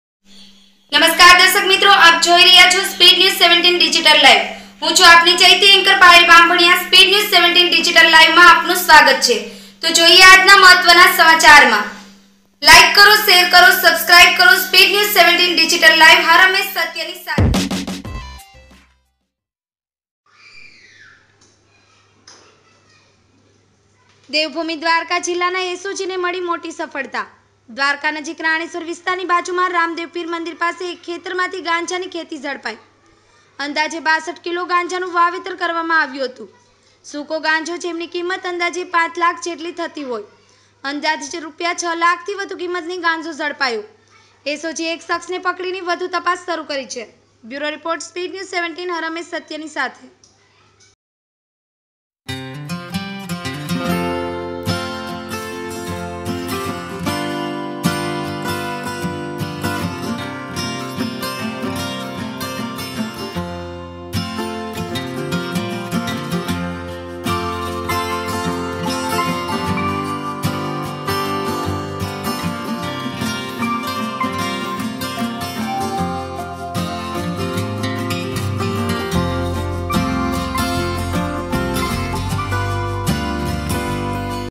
DWARKA દ્વારકા જિલ્લા SOG ને મળી મોટી સફળતા.
દેવભૂમિ દ્વારકા જિલ્લા SOG ને મળી મોટી સફળતા.

દ્વારકા નજીક રાણેશ્વર વિસ્તાર ની બાજુમાં રામદેવપીર મંદિર પાસે એક ખેતરમાંથી ગાંજાની ખેતી ઝડપાઈ.

અંદાજે 62 કીલો ગાંજાનું વાવેતર કરવામાં આવ્યું હતું.

સુકો ગાંજો જેમની કિંમત અંદાજે પાંચ લાખ જેટલી થતી હોય

અંદાજિત રૂપિયા 6 લાખથી વધુ કીમત નો ગાંજા ઝડપાયો.

SOGએ એક શખ્સને પકડીને વધુ તપાસ શરૂ કરી છે.